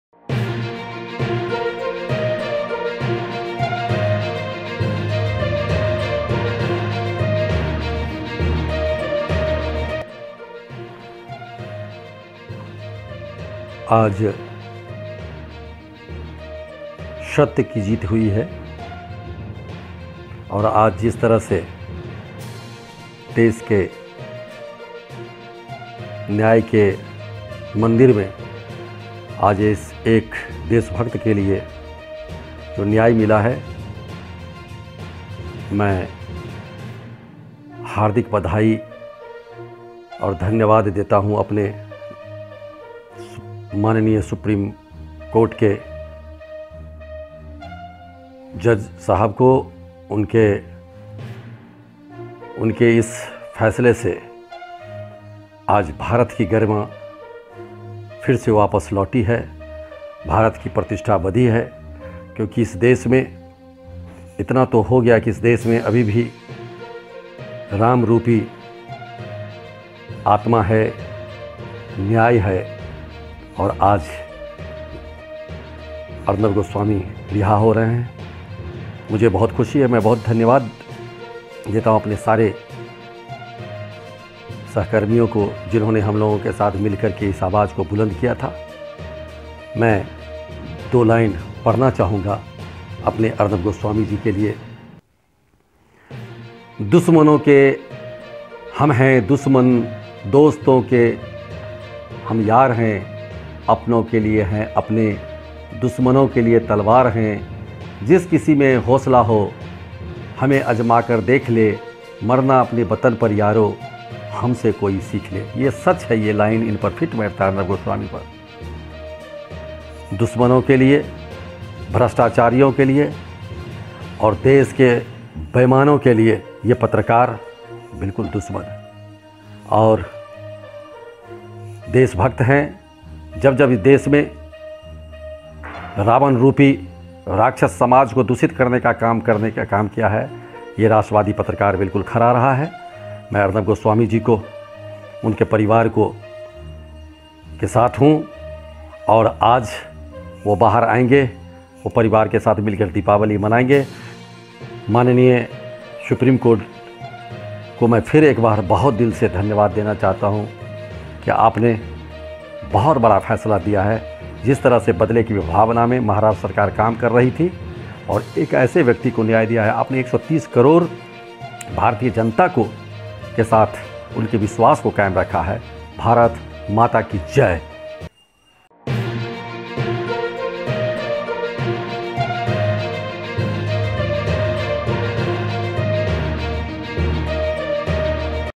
RC Singh Congratulate Arnab Goswami On his Bail from SC
News5 Bharat
arnabbail#rc singh#congratulationarnab